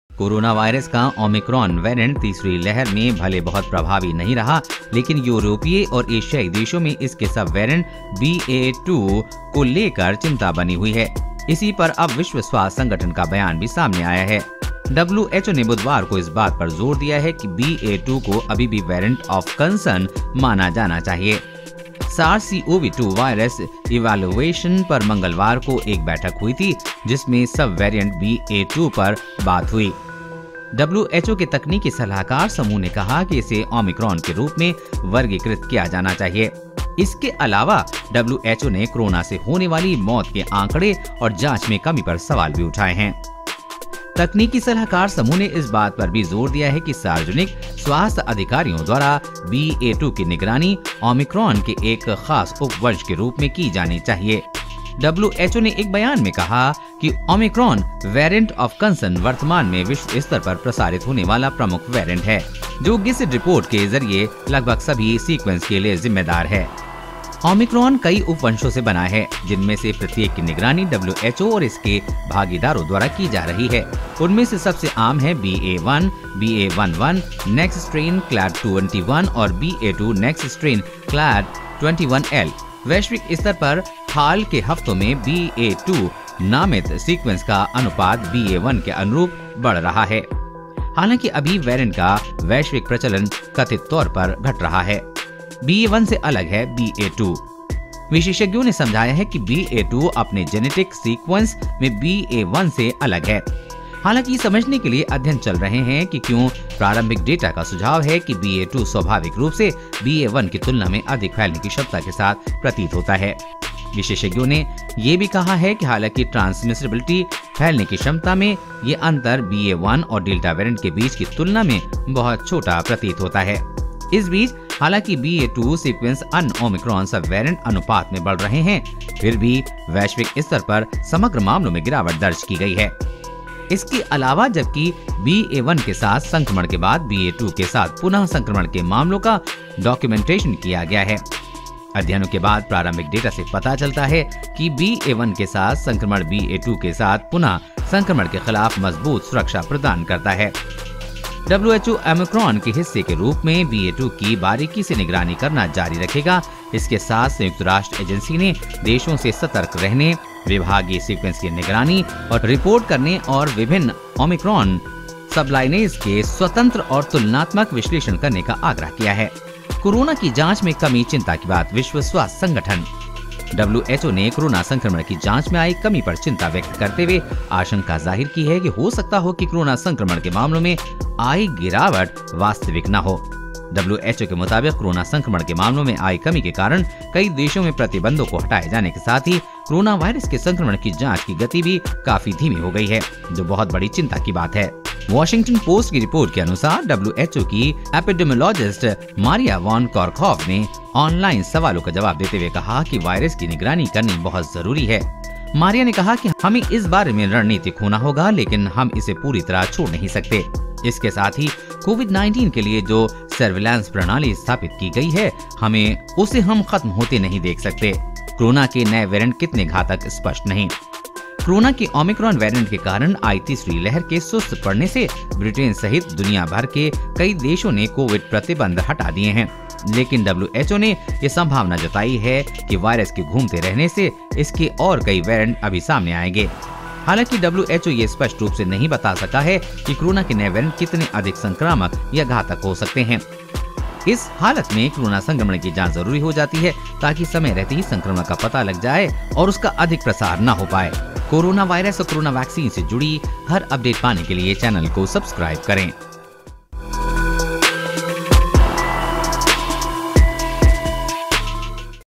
बड़ी खबर ! ओमिक्रोंन BA.2 वैरिएंट के खतरे को लेकर चिंता में WHO !
Who Speaks On Omicron Sub Variant Ba2 Said It Is Still A Variant Of Concern Also Raised Questions On Covid Test | WHO Concerned About Risk Of Omicron BA.2 Variant |  Coronavirus And Omicron Variant News And All Latest News Updates